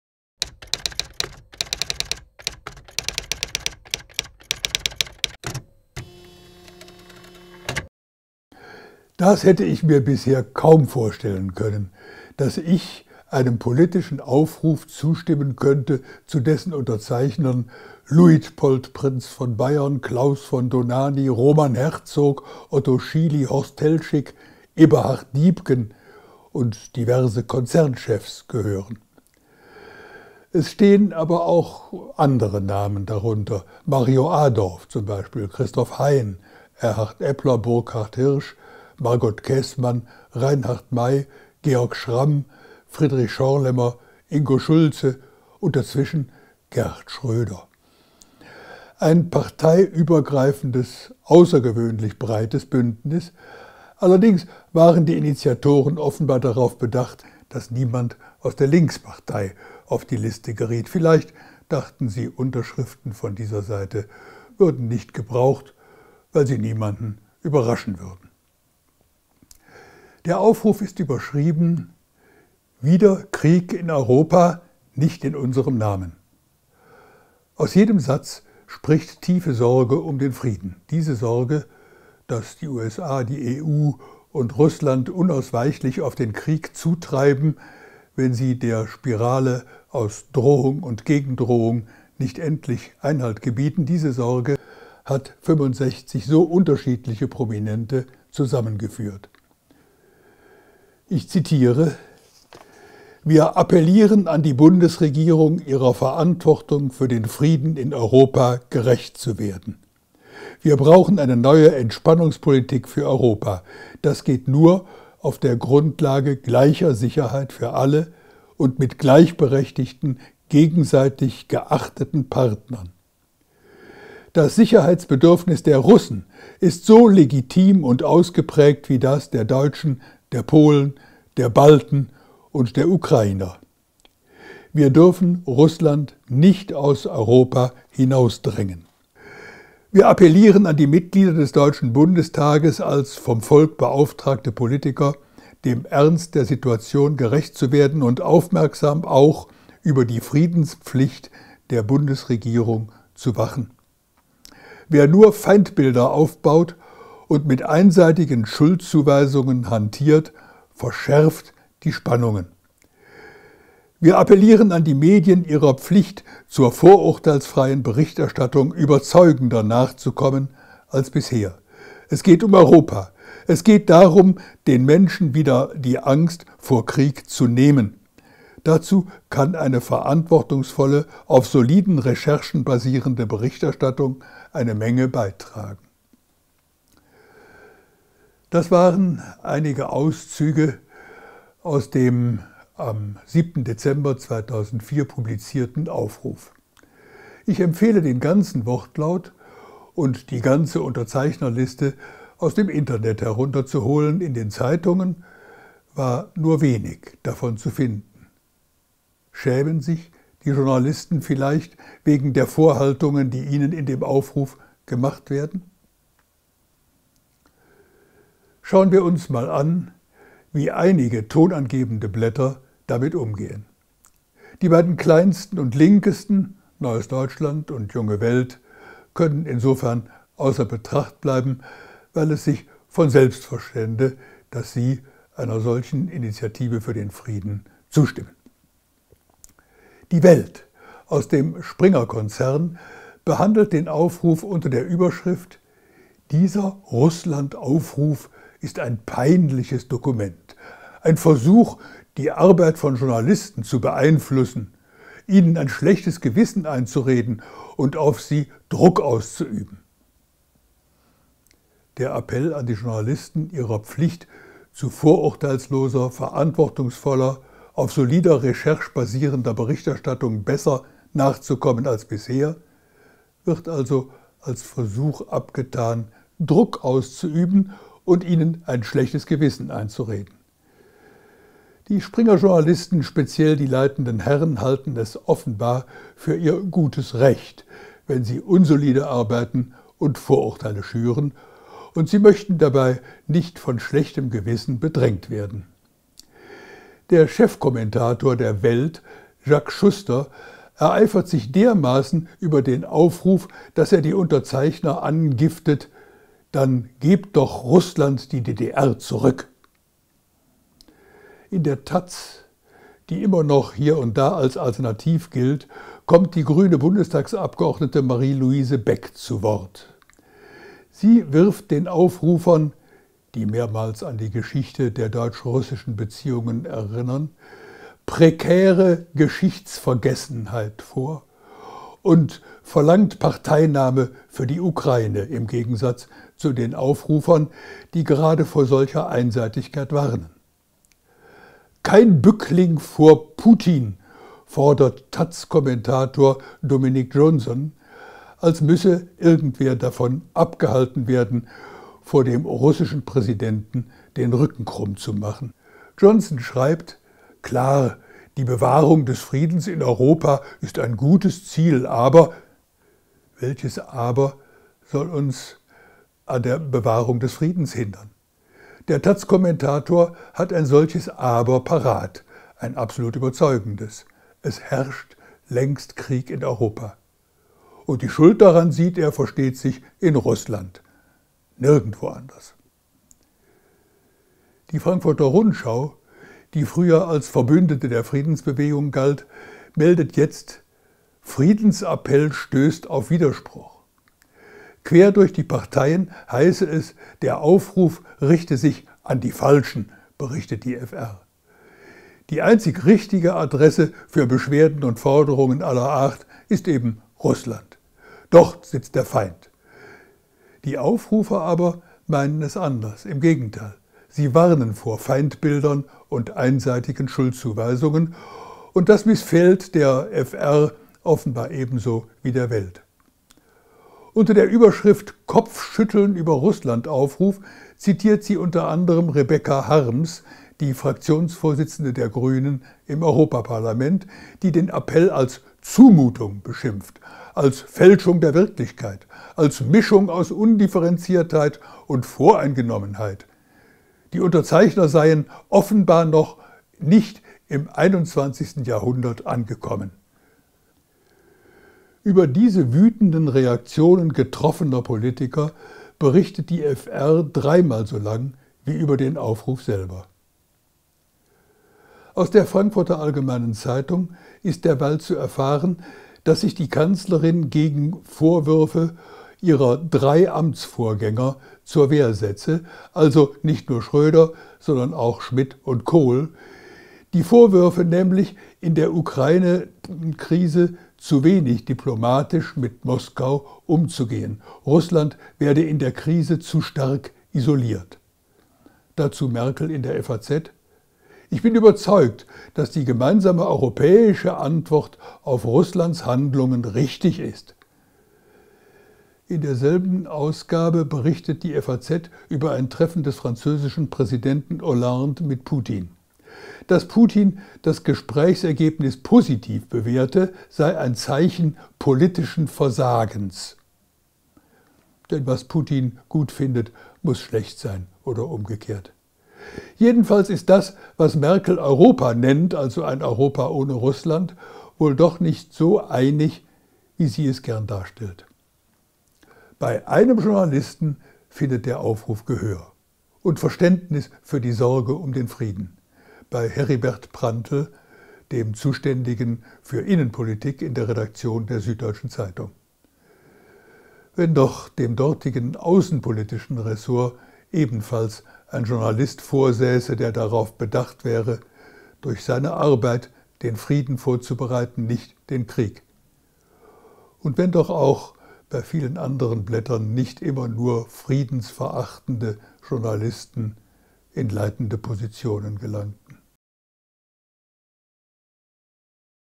Spoos Presseschau: Mediale Beißreflexe gegen Friedensaufruf
Die Reaktionen auf den Friedensaufruf „Wieder Krieg in Europa? Nicht in unserem Namen“

„Der Appell an die Journalisten, ihrer Pflicht zu vorurteilsloser, verantwortungsvoller, auf solider Recherche basierender Berichterstattung besser nachzukommen als bisher, wird also als Versuch abgetan, Druck auszuüben und ihnen ein schlechtes Gewissen einzureden, Nein, die Springer-Journalisten, speziell die leitenden Herren, halten es offenbar fuer ihr gutes Recht, wenn sie unsolide arbeiten und Vorurteile schüren und sie möchten dabei nicht von schlechtem Gewissen bedrängt werden.“